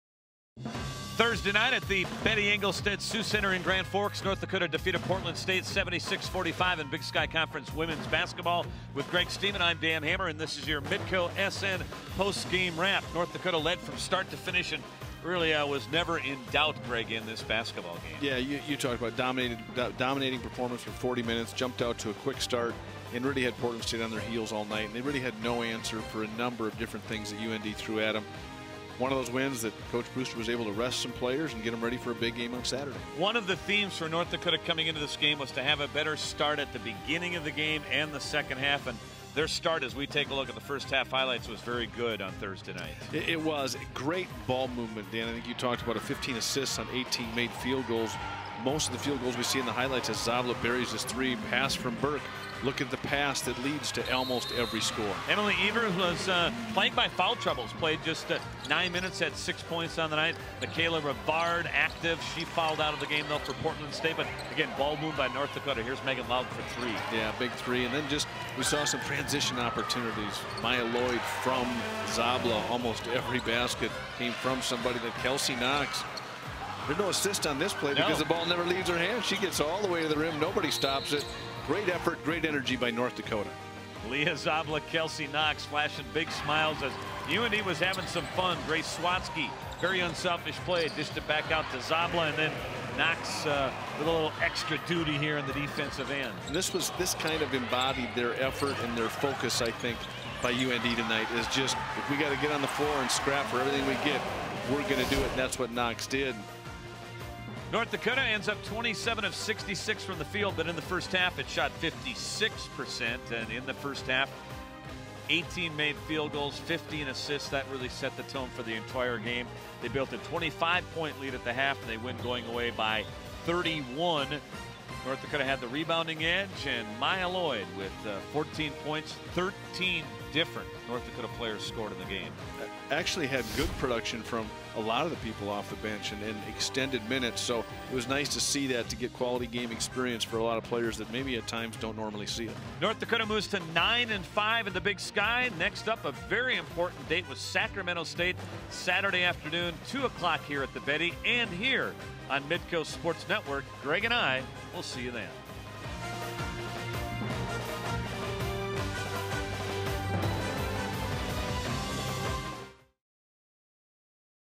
2.19.15 WBB Portland State at UND Post Game Wrap
UND takes control of the game early in a convincing win over Portland State. Dan Hammer and Greg Stemen have your Post Game Wrap.